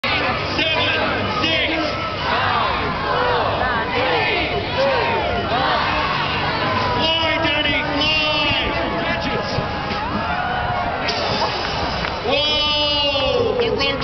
The men of the Birdman Rally: Part One
Those crazy cats at the Moomba Festival throw themselves into the Yarra for no good reason apart from proving their ability to design an aircraft are limited.